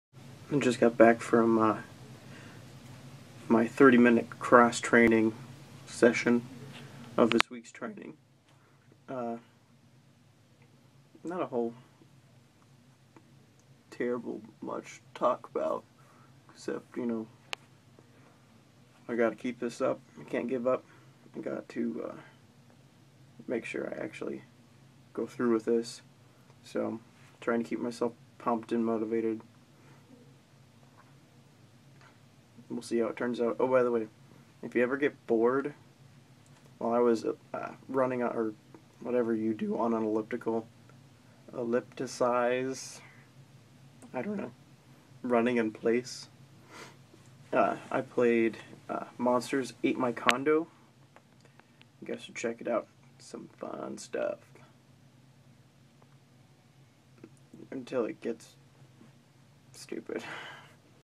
Half Marathon Training - Day 2
I "elliptisized" for thirty minutes today and played monsters ate my condo.